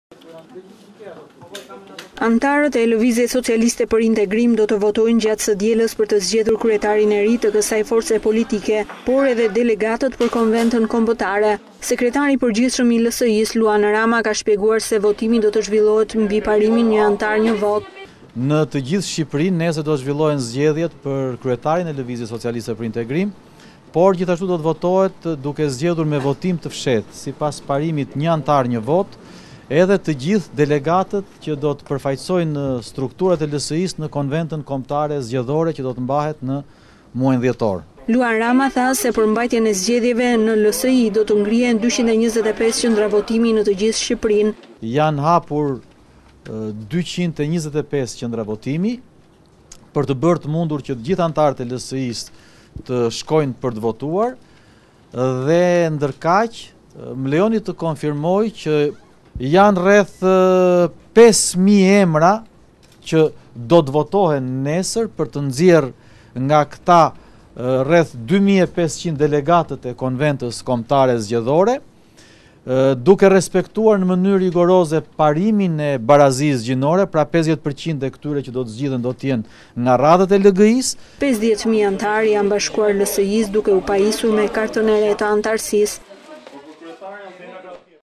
Ora News – Zgjedhjet në LSI, Rama: Zhvillohen mbi parimin një anëtar një votë
Lëvizja Socialiste për Integrim do të zhvillojë zgjedhje për kryetarin dhe delegatët për Konventën Kombëtare.Sekretari i Përgjithshëm i LSI-së, Luan Rama, ka sqaruar se në të gjithë Shqipërinë do të hapen 225 qendra votimi.
Anëtarët e Lëvizjes Socialiste për Integrim do të votojnë gjatë së dielës për të zgjedhur kryetarin e ri të ksaj force politike, por edhe delegatët për Konventën Kombëtare.
Sekretari i Përgjithshëm i LSI-së ka shpjeguar se votimi do të zhvillohet mbi parimin një anëtar një votë.
Rama: Në të gjithë Shqipërinë nesër do të zhvillohen zgjedhjet për kryetarin e LSI-së, por gjithashtu do të votohet duke zgjedhur me votim të fshehtë, sipas parimit ‘një anëtar një votë’, edhe të gjithë delegatët që do të përfaqësojnë strukturat e LSI-së në Konventën Kombëtare Zgjedhore që do të mbahen në muajin dhjetor.
Luan Rama ka shpjeguar se për mbajtjen e zgjedhjeve  në LSI do të ngrihen 225 qendra votimi në gjithë Shqiperine.